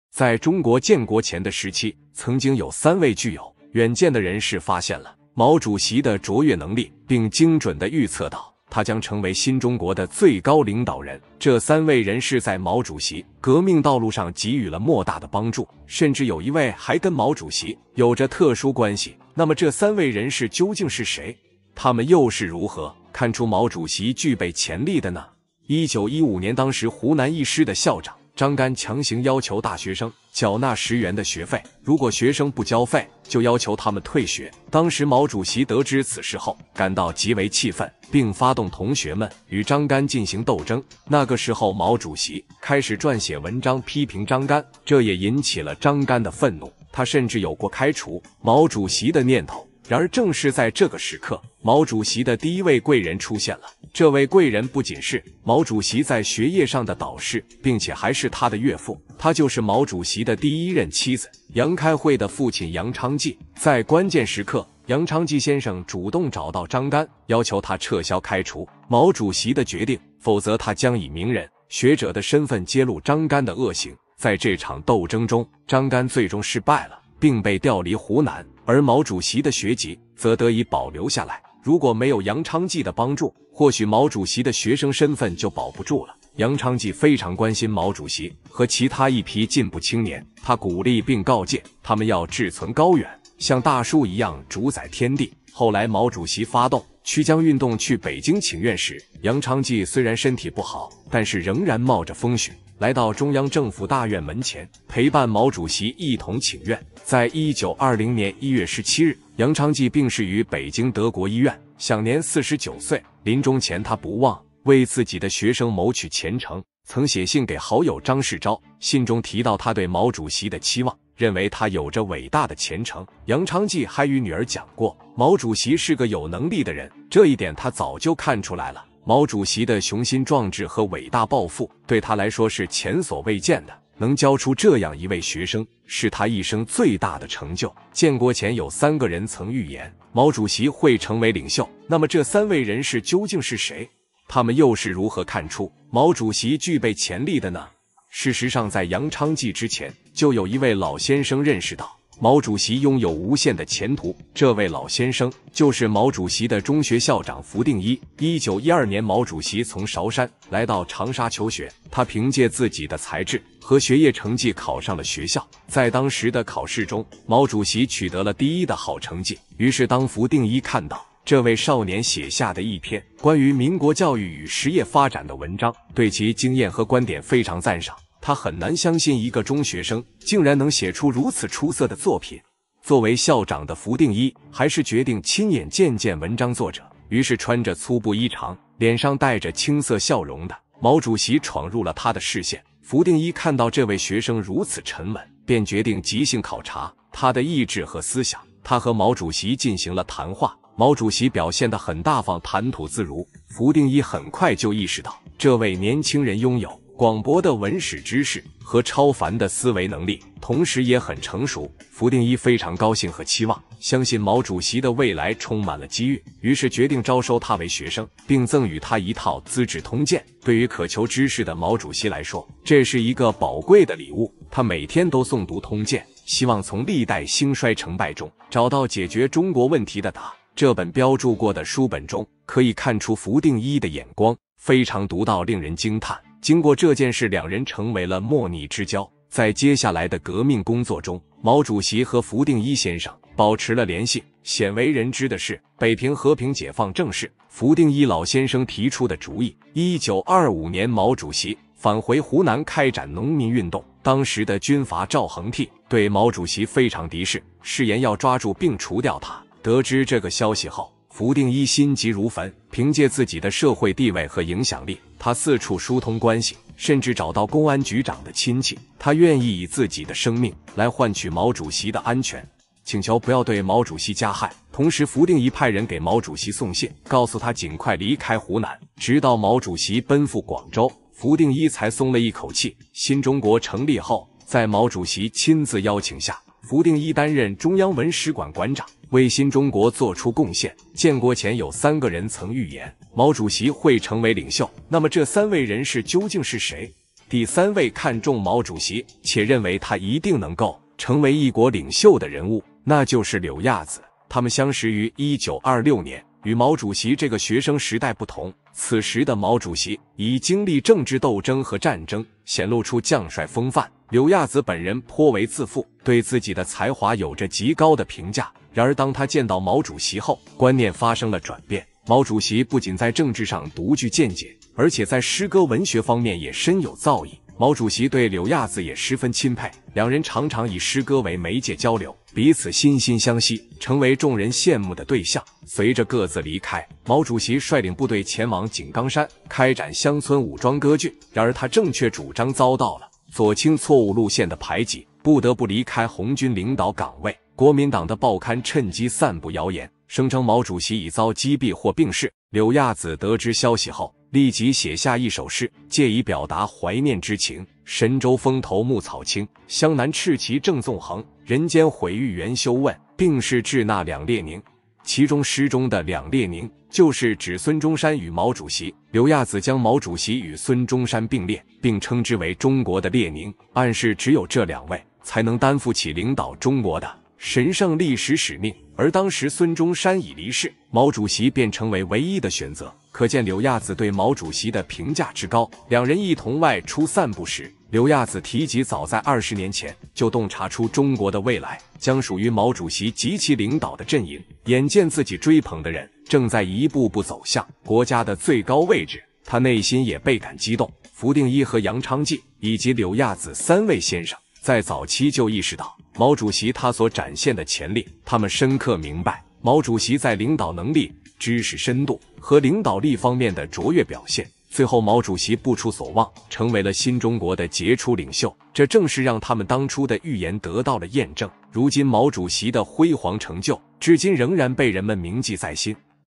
他們三人最早預料到，毛主席將成為中國領袖，這三個人到底是誰？
No.1 | 湖南12歲男孩精通鳥語，憑特殊語言指揮鳥兒做事，被村民稱為鳥神